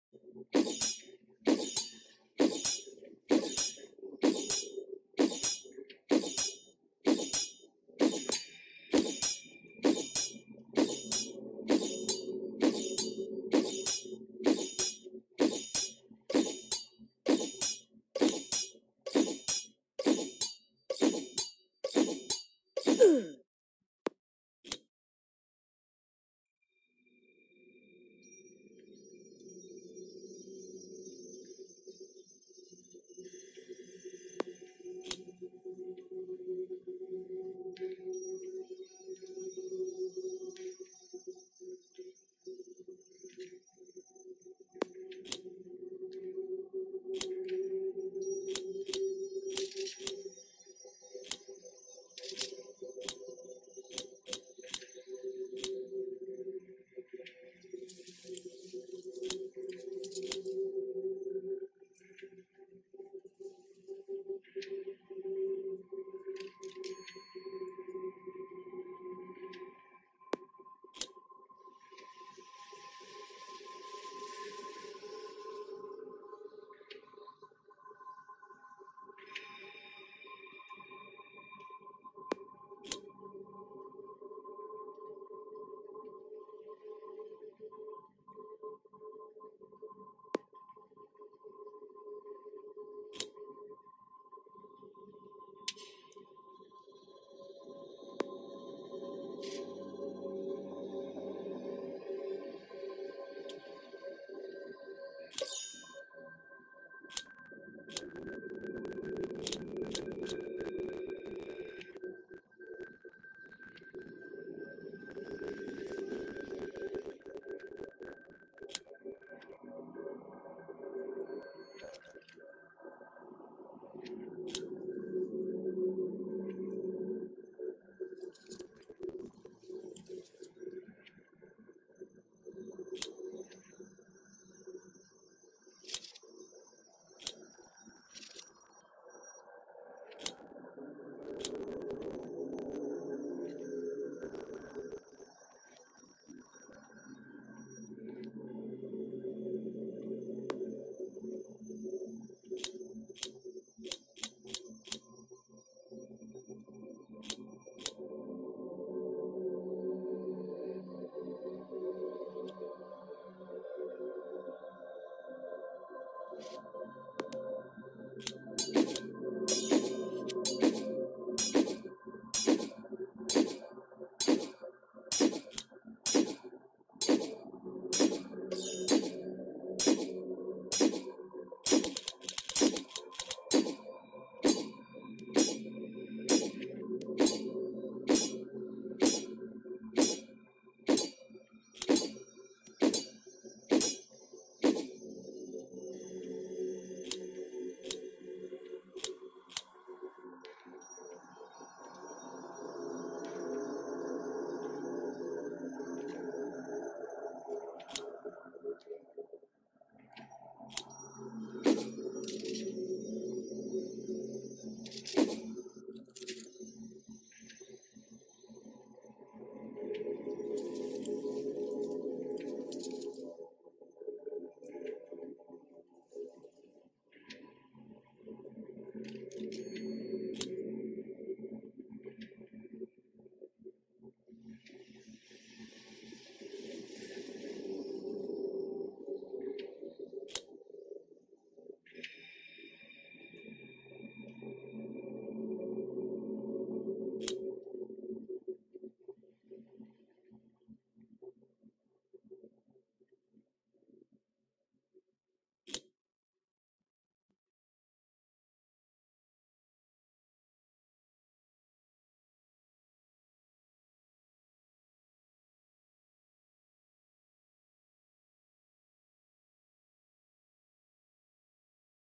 Bring 5 spectre dusts and 20 blitzbee stings to the robbed knight
next video created by my son and me for ft community !

in this video we shown the reward on a quest in the dungeon under kings garden !

for the quest we needed to drop 5 spectre dusts from the spectre mages living in the dungeon of doom behind the maze (i`ve shown u in other video the walktrhrough the maze and where the spectre mages live).
And we needed to drop 20 blitzbee-stings.
Blitzbees living on bees island (map -1,-3), or on map 1,3 !
to reach bees island u have to do a quest too.
maybe if i get little more time i will show u on other acc how to reach bees island !
For faster reaching this level my mage helps a bit to drop the needed items for this quest !

Thx for watching the video.

The reward this quest is +1 level, and getting next level (bring 2 chaotic armors lvl 250). They needed to b crafted, or found in a ches, bought from other players, or 1 u wear on yourself if u 250+ :)

now it will take  a little time, because i will craft that plates !

What is needed to craft them it will shown u in a craft menu in house or clan !

Have fun nd hopefully seen again in my (our) nxt video !

Greetings...SK